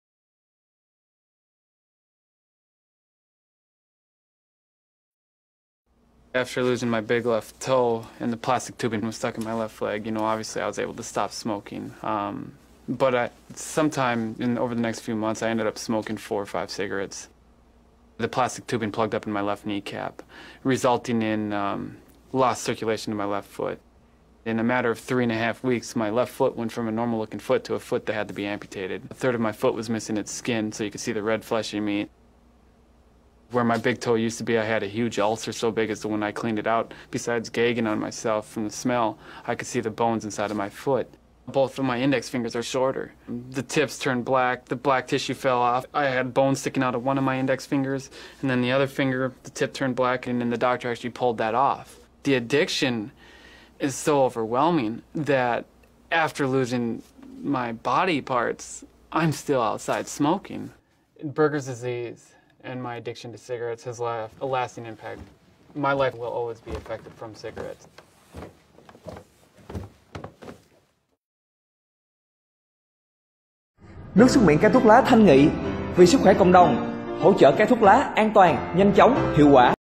Mất đôi chân vì thuốc lá
Nam thanh niên đã vĩnh viễn mất đi đôi chân của mình vì thuốc lá.